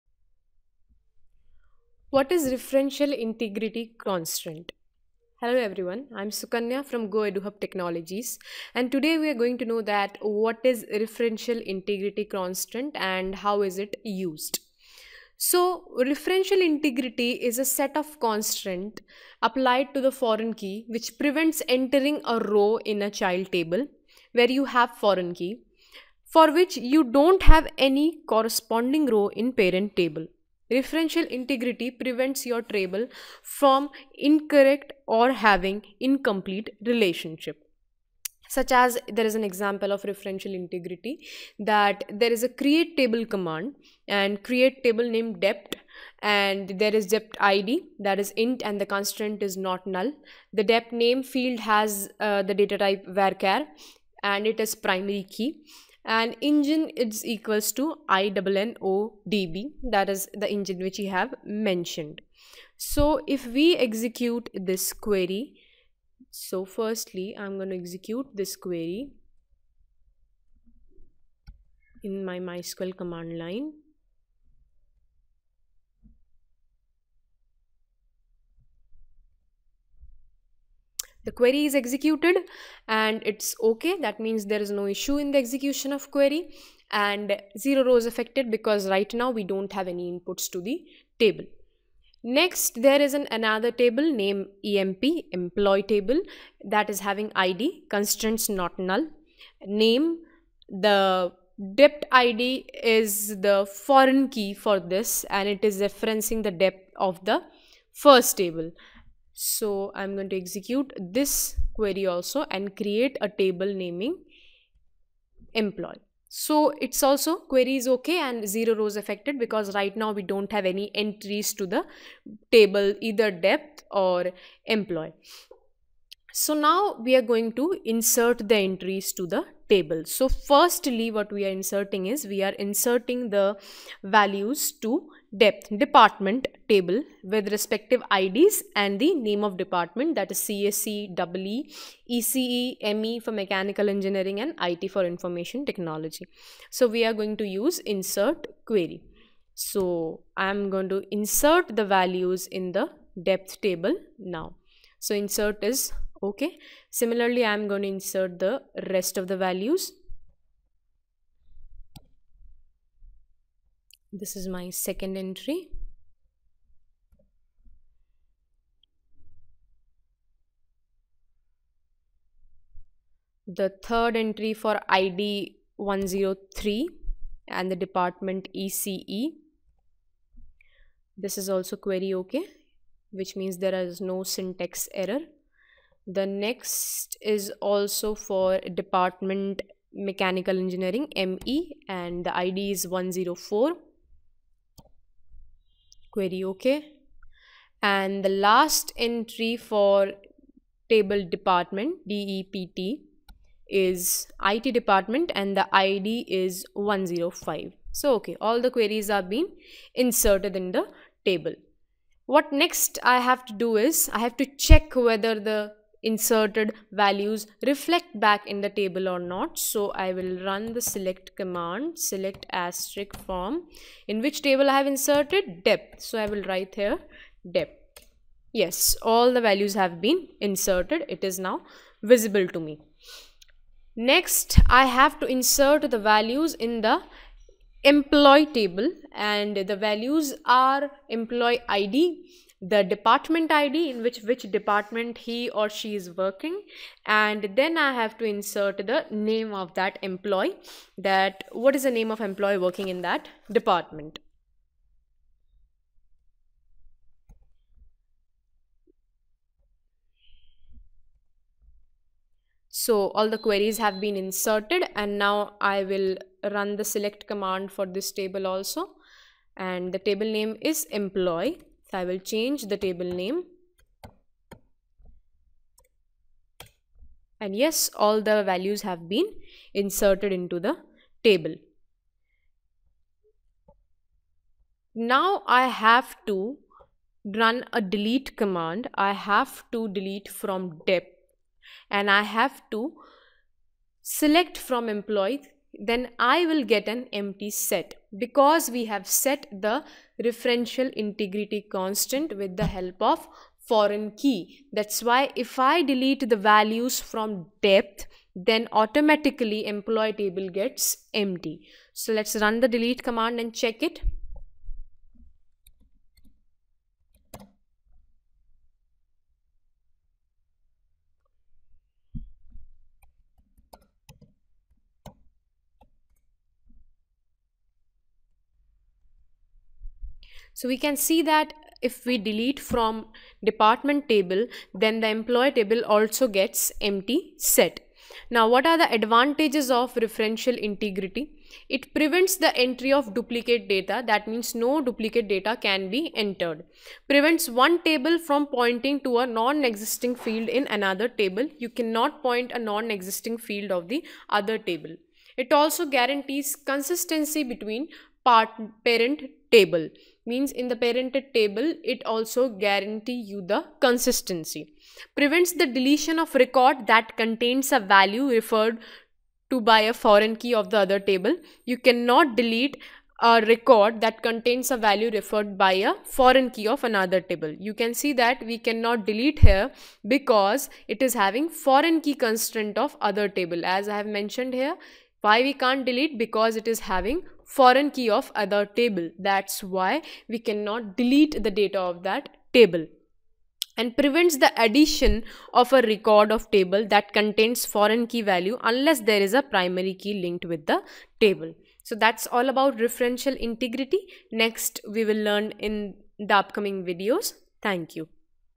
Using the referential integrity constraints.
Referential Integrity Constraint :  Referential Integrity is set of constraints applied to foreign key which prevents entering a row in child table (where you have foreign key) for which you don't have any corresponding row in parent table .Referential Integrity prevents your table from having  incorrect or incomplete relationship .